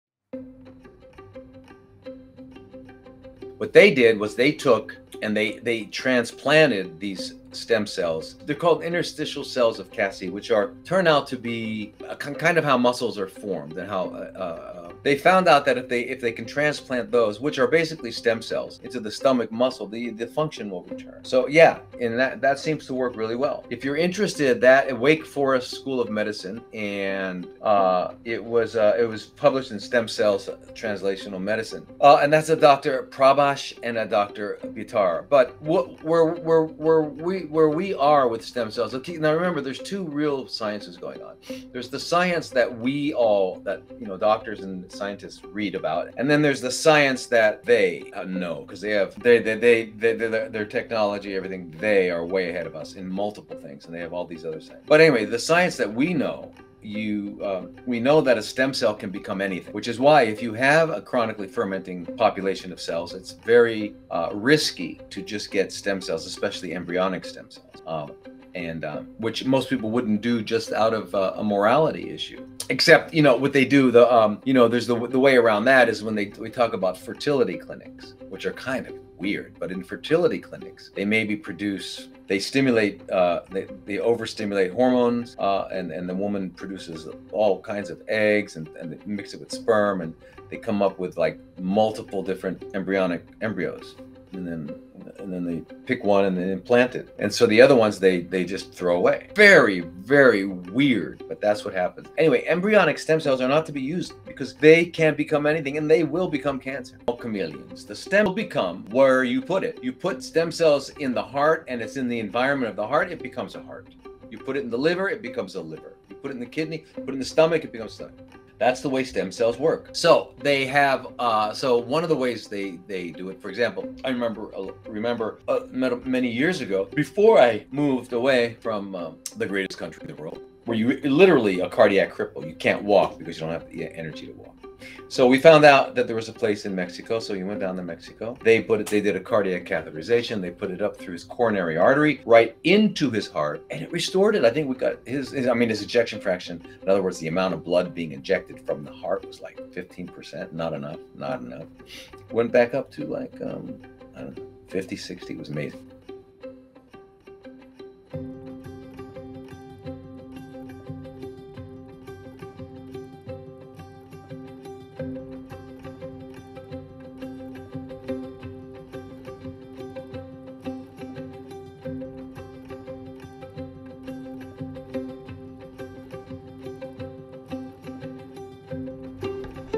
Thoughts on Stem Cells - Dr. Lodi
Thoughts on Stem Cells - In this video Dr. Lodi talks about stem cells!